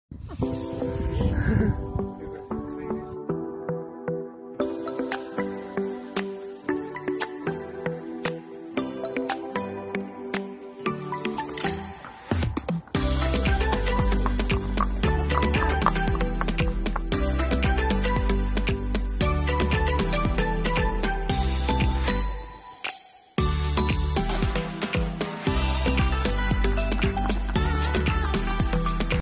A girl and her Pony
Just a little edit of Clover and Daisy from her first ever sit on her to now ❤